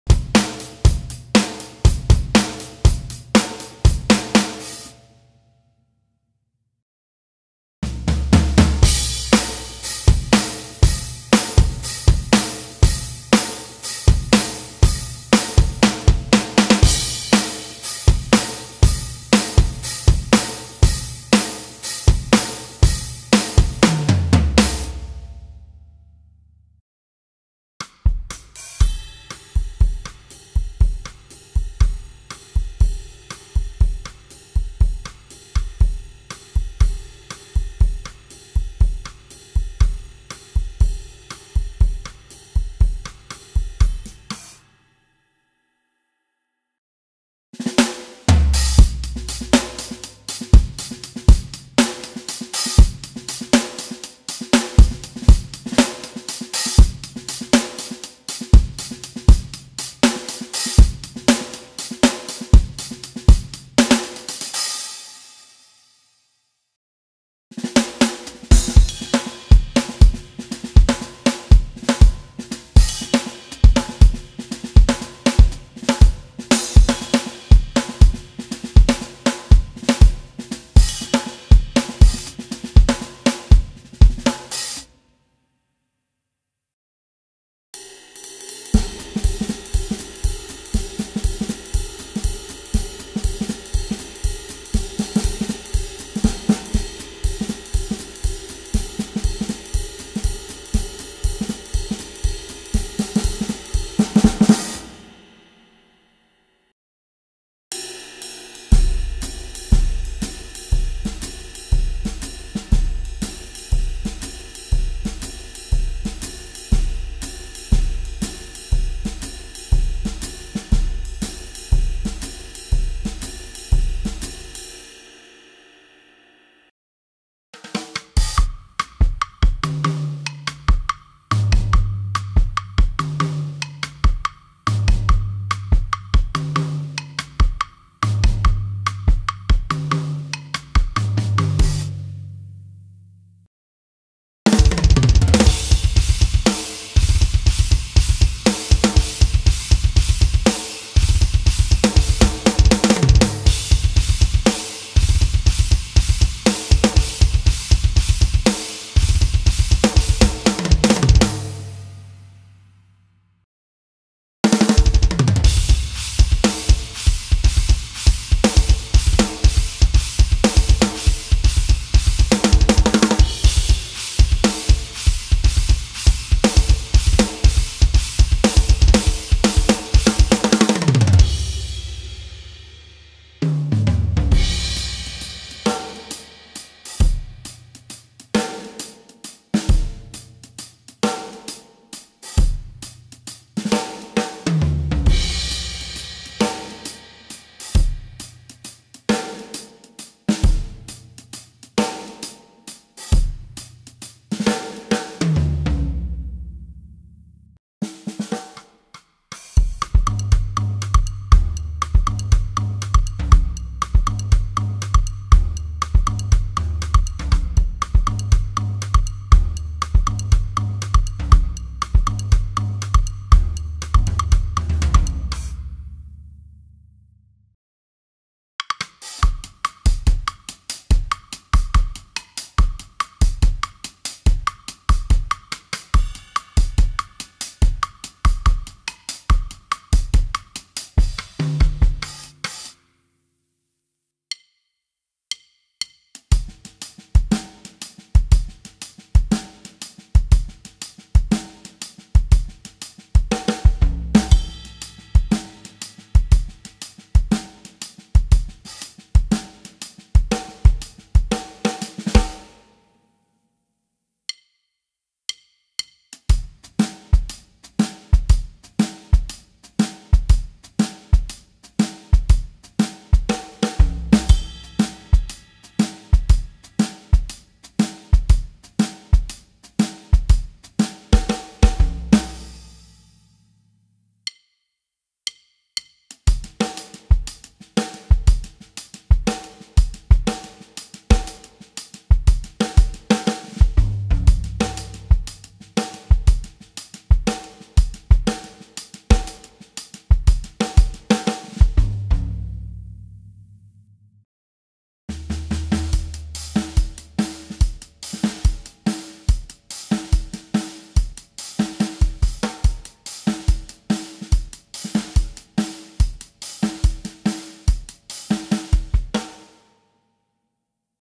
Free Drum Beats - Instrumental Loops For Songs - No Copyright Download
Download these free drum beat instrumentals which are loop ready for songs of many genres. No copyright at all! Various tempos and styles on the website free for download.

Hi, my name is David Fesliyan and I'm a music composer from California.